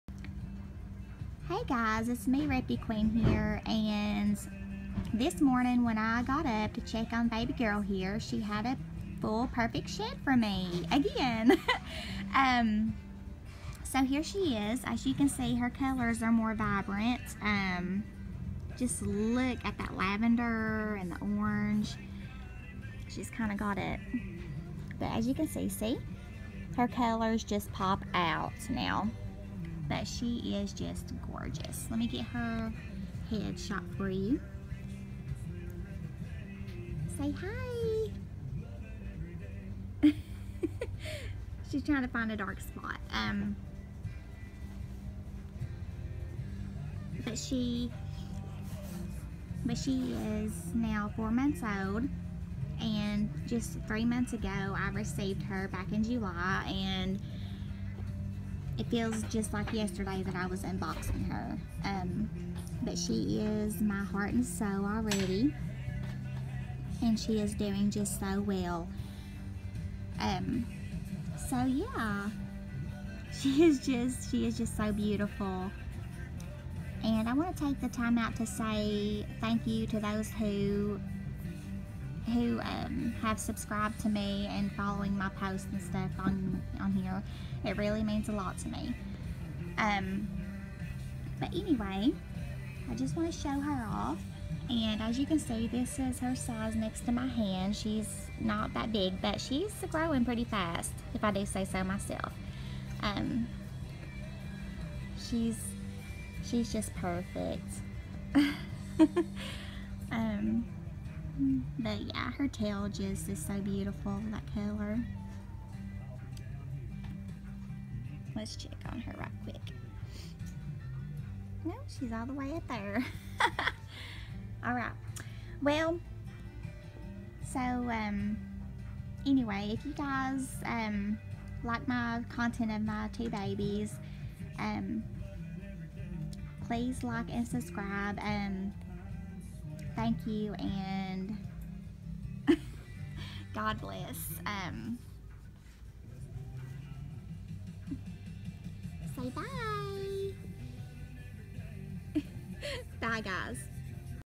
Chi-chi's NEW Paint Job This Morning! 😍🐍💕
Hi guys! This morning when I woke up & checked on my baby girl, she had a full shed for me over night! She looks better and amazing! She is my whittle princess and my heart ❤️ and soul. Thank you and God bless each and every one of you, 😊